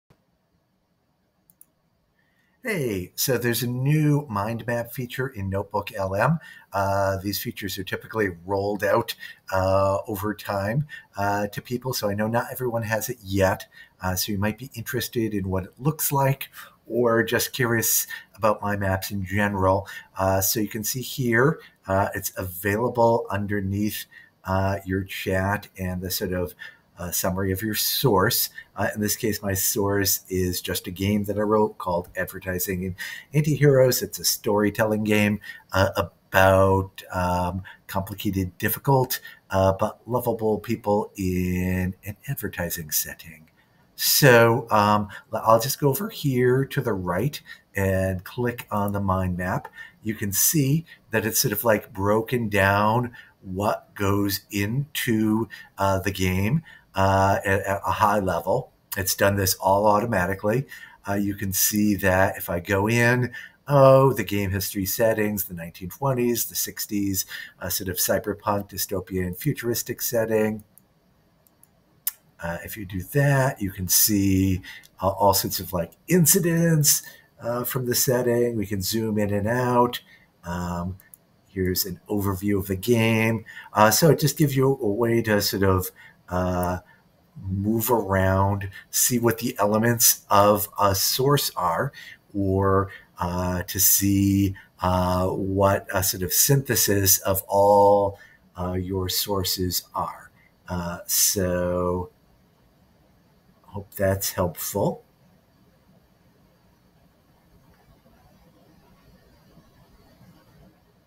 Quick overview of the new Mind Map feature in NotebookLM
I'm showing how the Mind Map feature in NotebookLM works.